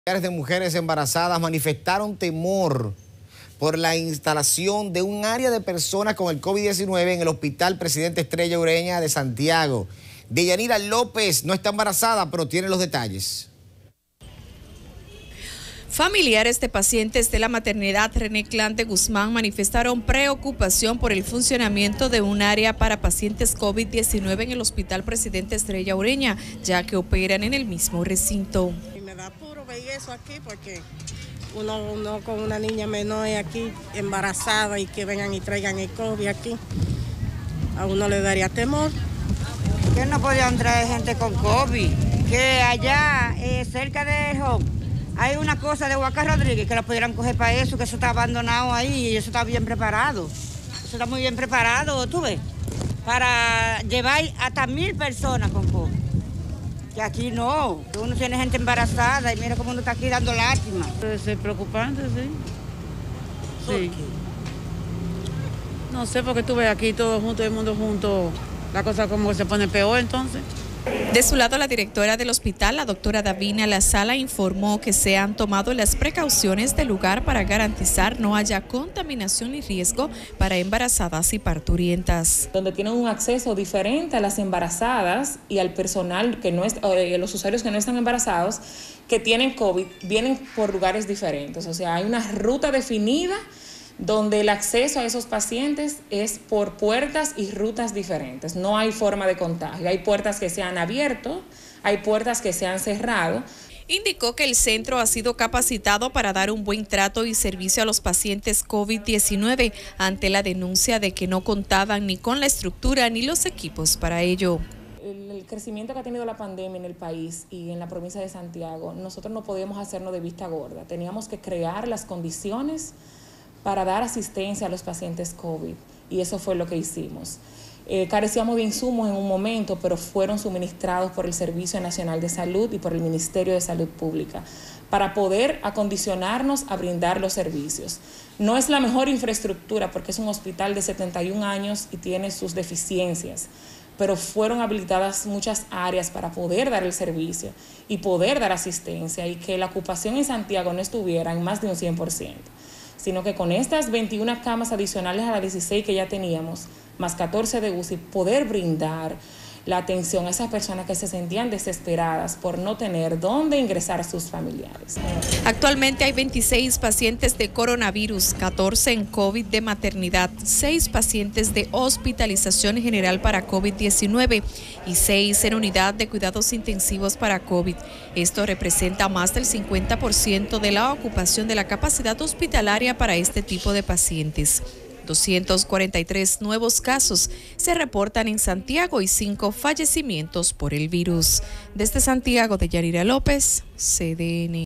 Familiares de embarazadas manifiestan temor por instalación de área Covid en hospital en Santiago
Familiares de pacientes embarazadas manifiestan temor por instalación de área en hospital Presidente Estrella Ureña